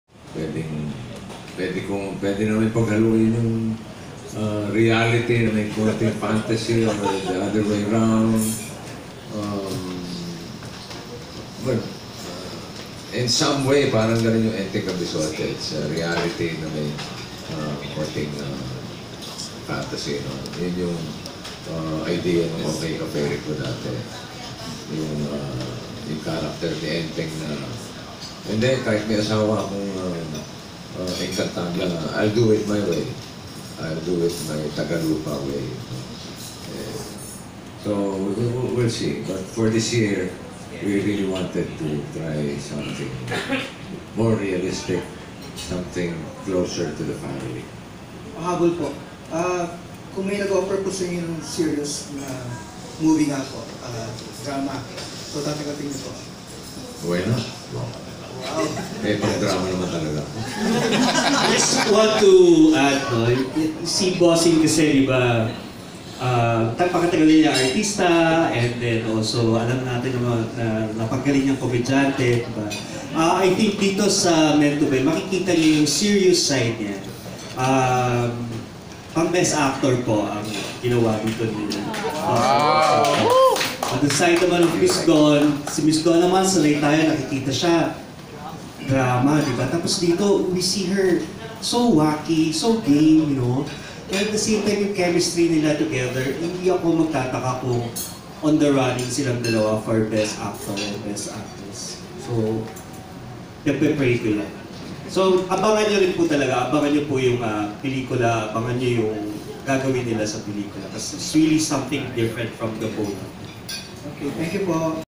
Meant To Beh: Bossing Vic Sotto as Drama Actor and Dawn Zulueta as Comedy Actress
Coverage of the Meant To Beh Blogcon